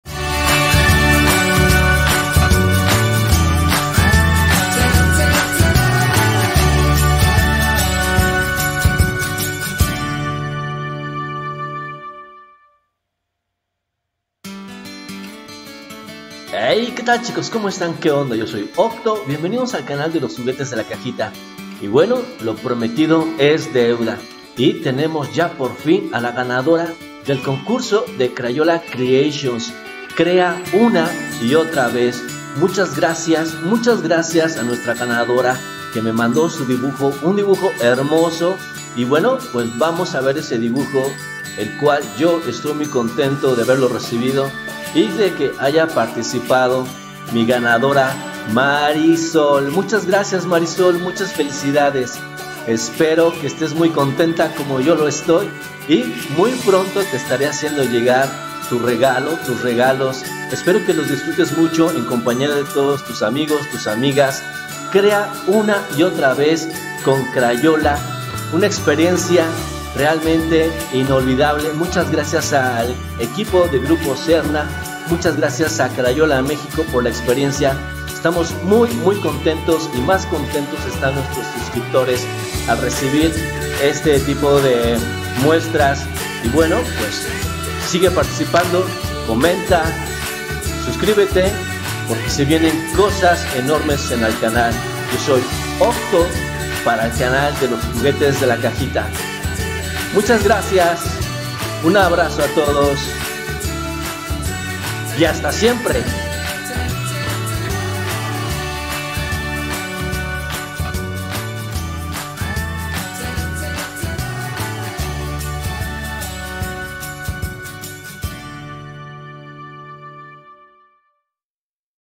🎨 FASHION DAY de Crayola: ¡Donde la Moda se Encuentra con el Color! 🌟
Prepárate para una explosión de creatividad y estilo en el FASHION DAY de Crayola. Este evento único celebra la fusión entre la moda y el arte, destacando cómo los colores pueden transformar y dar vida a cualquier look. Desde desfiles de moda hasta talleres interactivos, Crayola te invita a explorar el mundo de la moda a través de una paleta infinita de colores.

👗 Lo que podrás disfrutar:

Pasarelas con los diseños más innovadores, creados con productos Crayola.
Talleres donde podrás diseñar y personalizar tu propia ropa.
Charlas inspiradoras con diseñadores y artistas del color.
Actividades creativas para todas las edades, desde niños hasta adultos.
✨ No te pierdas:

Presentaciones especiales y sorpresas exclusivas.
Concursos de diseño con increíbles premios.
Oportunidades para descubrir nuevas tendencias y llevarte a casa recuerdos únicos.

VIDEO APTO PARA TODO PUBLICO, POR LO QUE NO DEBE CONSIDERARSE COMO "CONTENIDO PARA PÚBLICO INFANTIL"

Crayola México